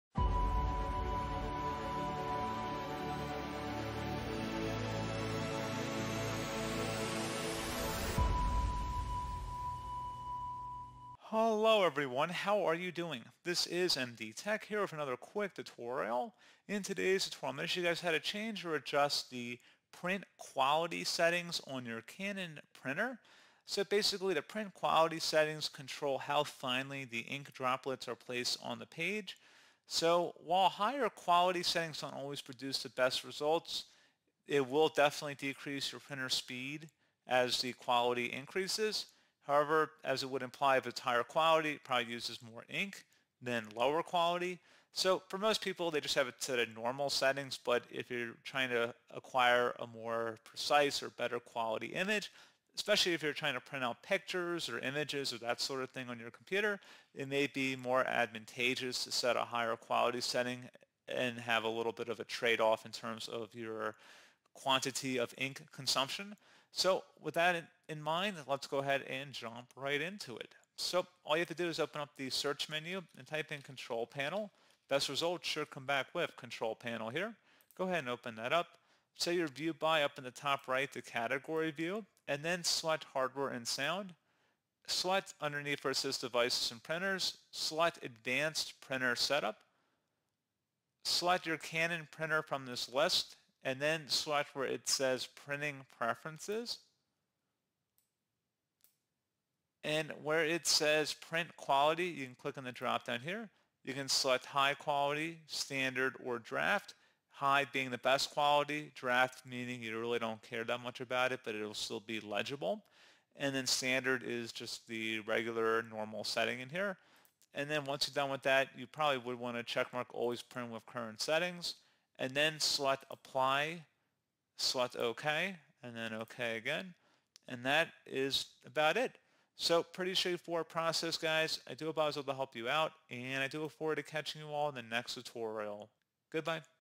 How to Change Print Quality on Canon Printer [Guide]
Learn how to change print quality on your Canon printer with our comprehensive guide. In this tutorial, we provide step-by-step instructions to help you adjust the print quality settings according to your printing needs. Whether you're printing documents, photos, or graphics, customizing the print quality ensures optimal results, whether you prioritize speed or high-resolution output.